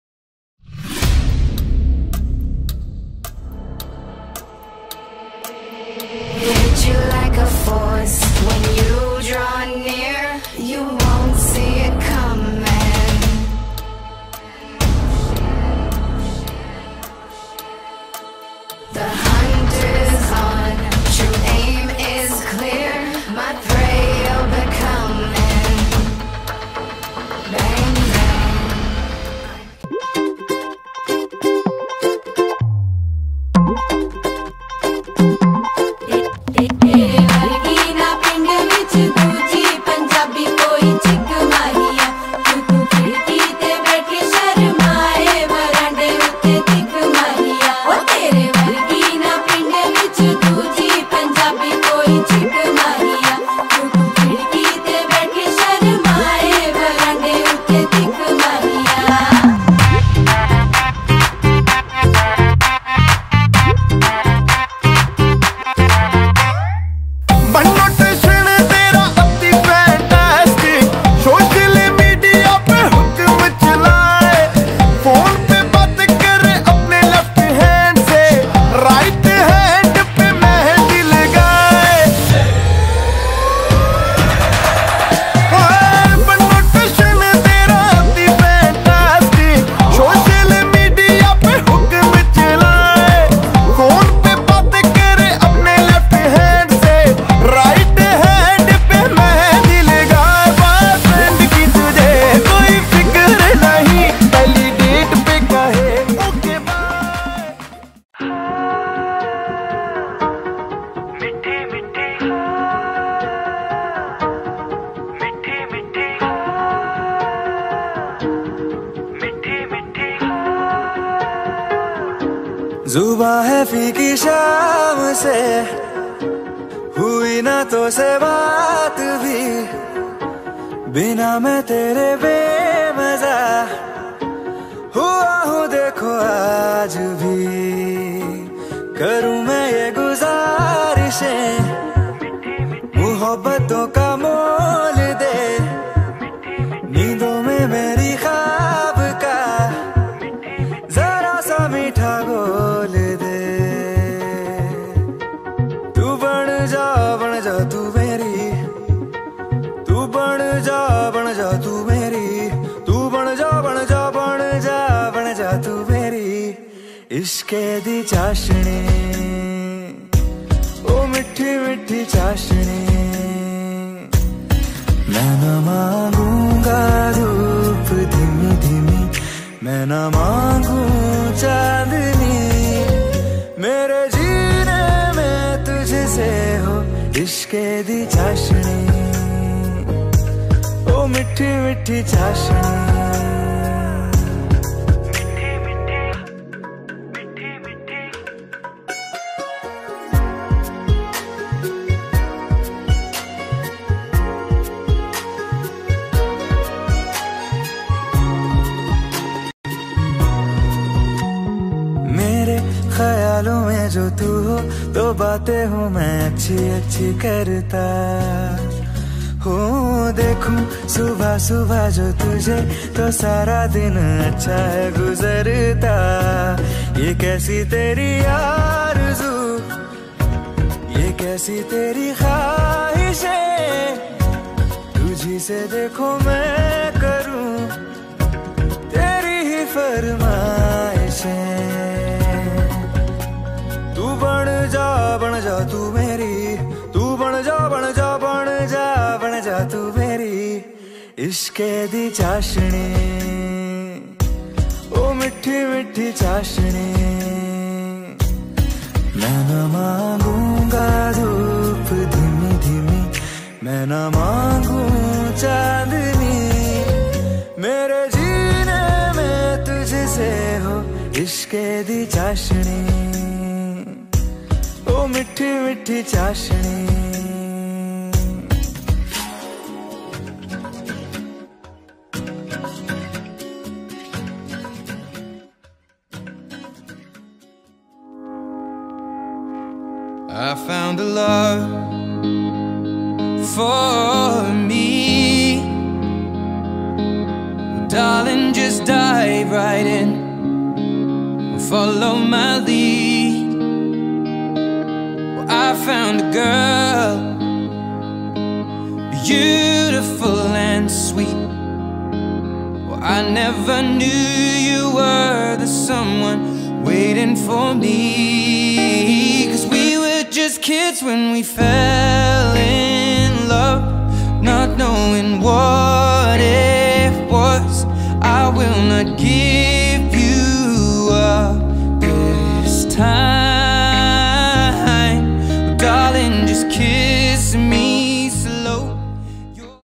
OUR WEDDING SHOWREEL | Tanay & Mallika
Presenting our Wedding Showreel.

Do shower your love & blessings.

Sangeet: 21st January 2022, Jayaleela Banquets, Mumbai
Wedding & Reception: 23rd January 2022, Goldfinch Hotel, Mumbai

My Vlogging Setup:
Action Camera
Mic 1
Mic 2
Wireless Mic
My Mobile